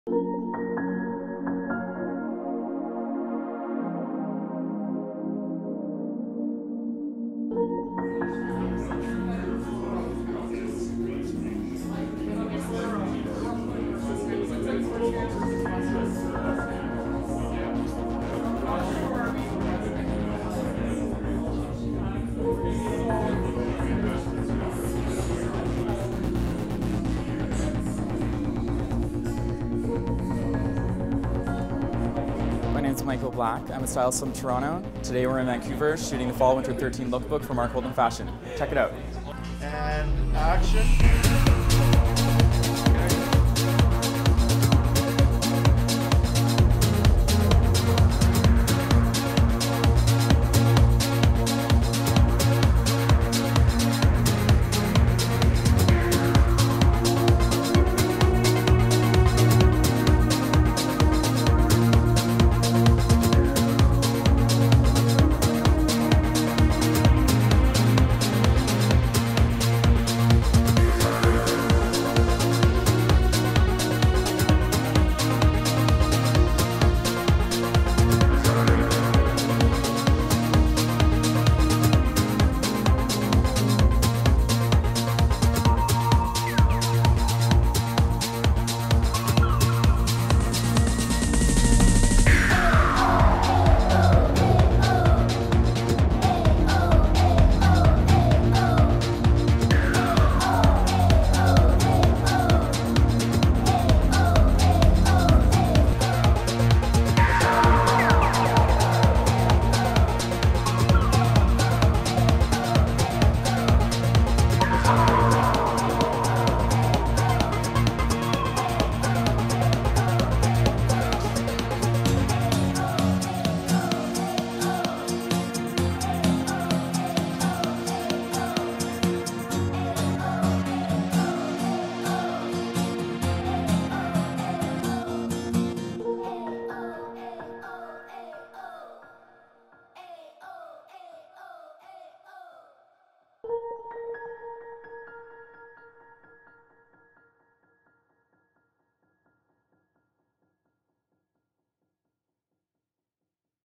Behind The Scenes - Mark Holden Fashion Shoot
Take a look behind the scenes of a fashion photo shoot for a beautiful collection by Vancouver designer, Mark Holden.

Faye Smith Makeup & Hair
Mark Holden Fashion
Brandon Hart Photography
and more...